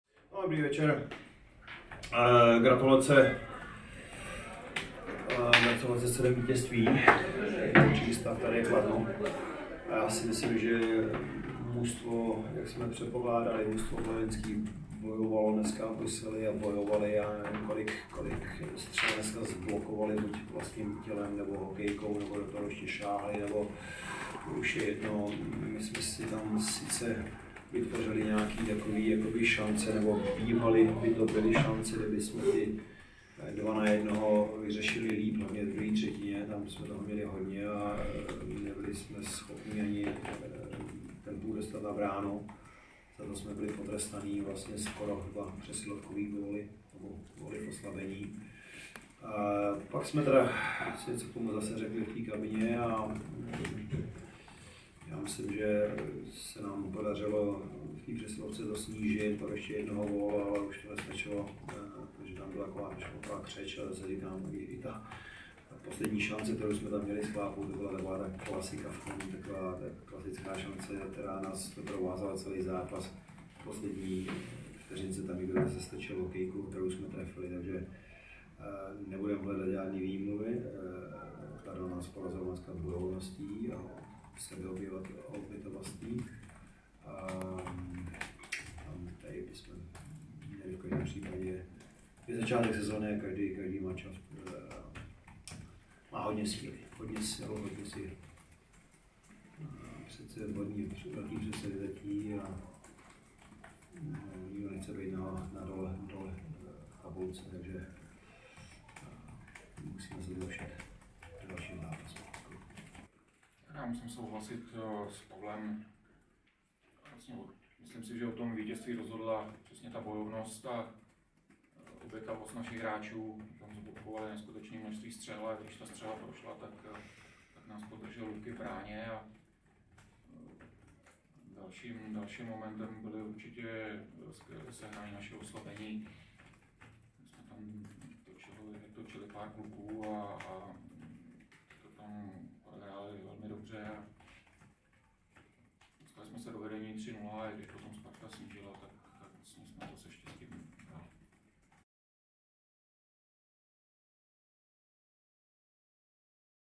Tisková konference po utkání 2. kola Tipsport extraligy, 20. 9. 2024
Trenéři po zápase: Rytíři Kladno - HC Sparta Praha: David Čermák a Pavel Gross